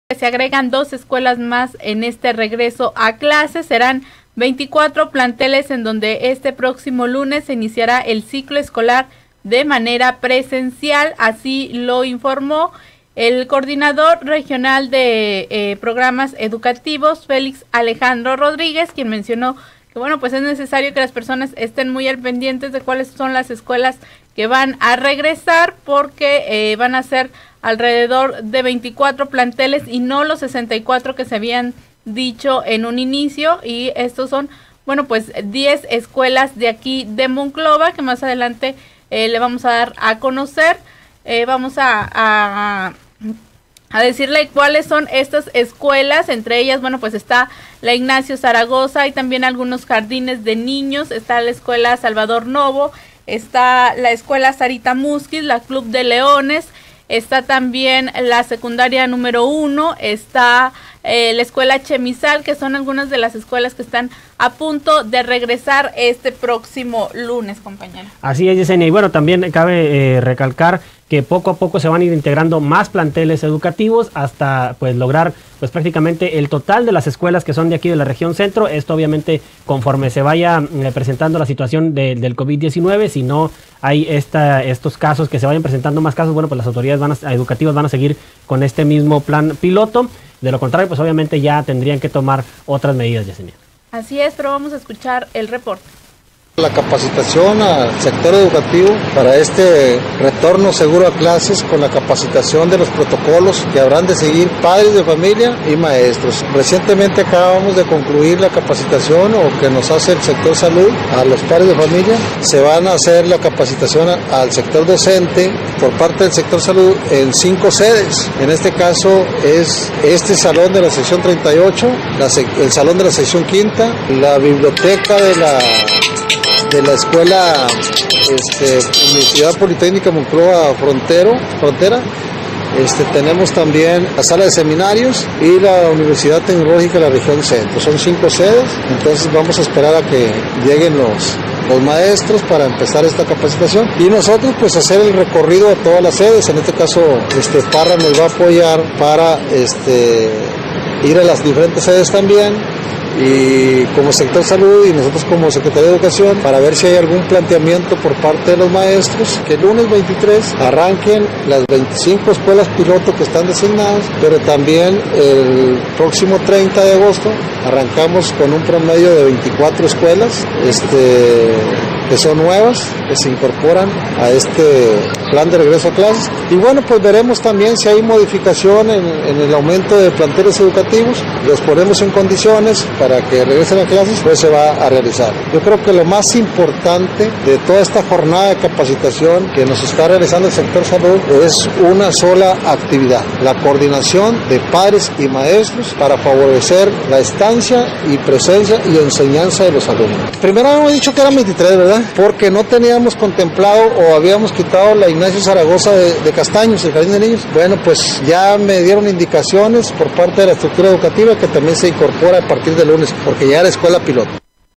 Monclova Se agregarán dos escuelas más en este regreso a clases, serán 24 planteles.
Monclova Se agregarán dos escuelas más en este regreso a clases, serán 24 planteles en donde este próximo lunes se iniciará el ciclo escolar de manera presencial, informó Félix Alejandro Rodríguez quien es coordinador regional de programas educativos.